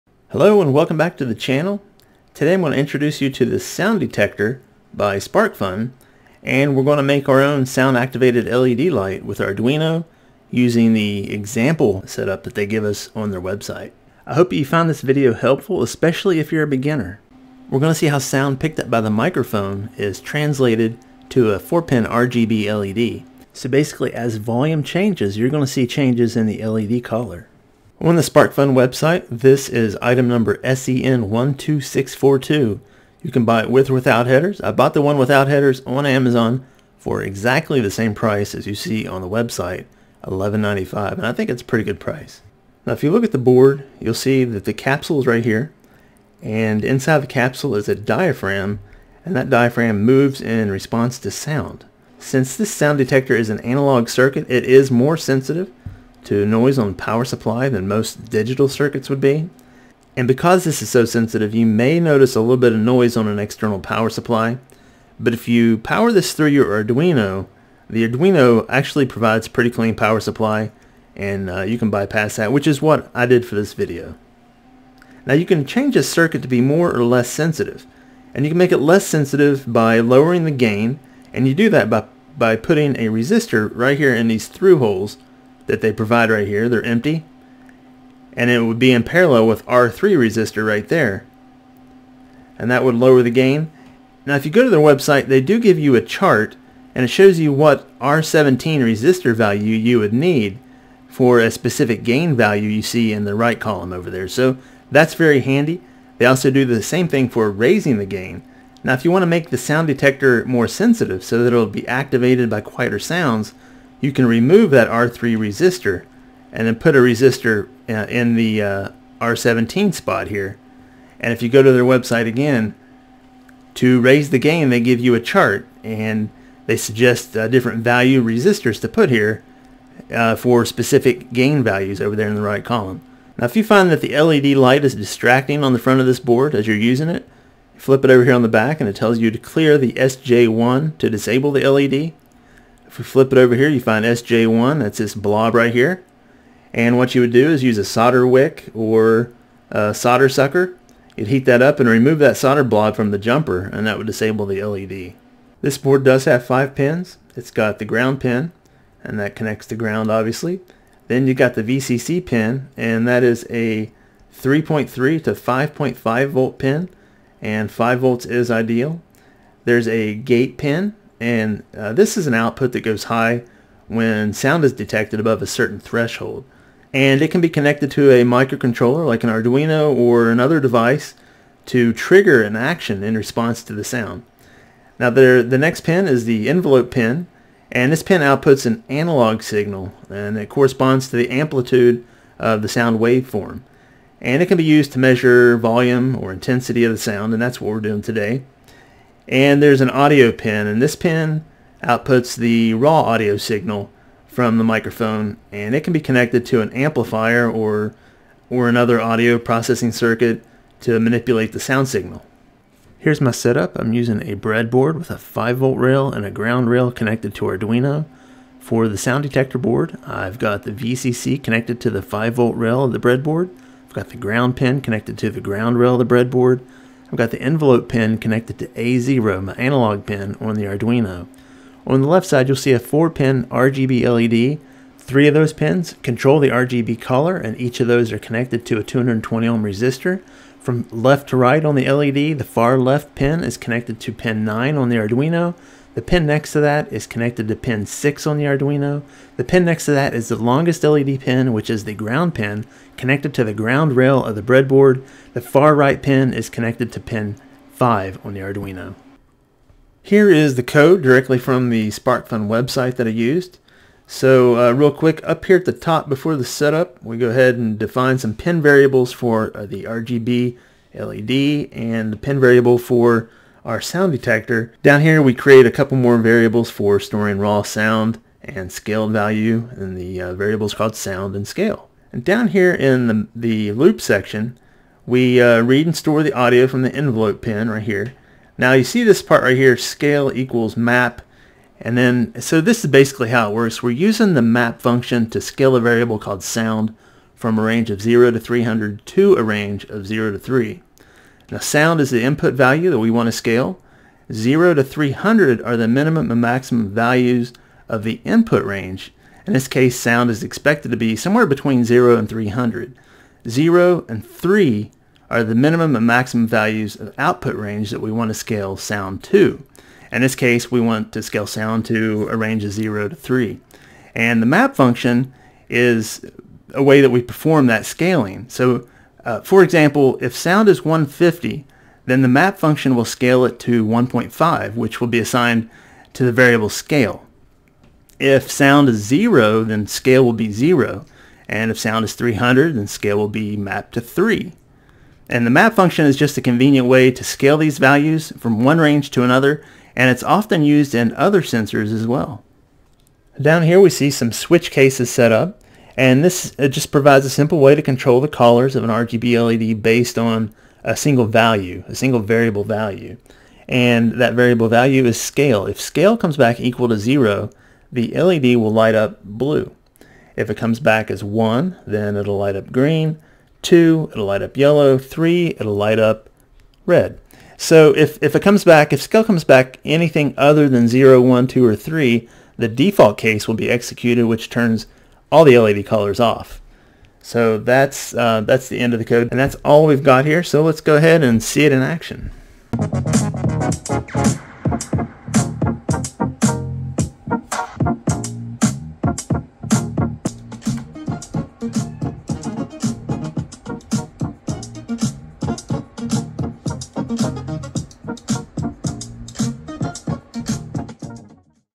Make Your Own Sound Activated LED Light With Arduino Sound Detector - A Beginner Video
Check out this beginner video and make your own sound activated LED light display using a sound detector and Arduino. This is a Sparkfun sound detector board and the setup at the end of the video is from their website.

Code is on the Sparkfun website (see link below). I'll also post it to our Facebook page - just search leduino to find it on Facebook.

Chapter:
0:00 intro
0:28 The Sparkfun Sound Detector
2:35 pinout
3:32 wiring connections
4:19 Arduino IDE code
6:32 sound-activated led in action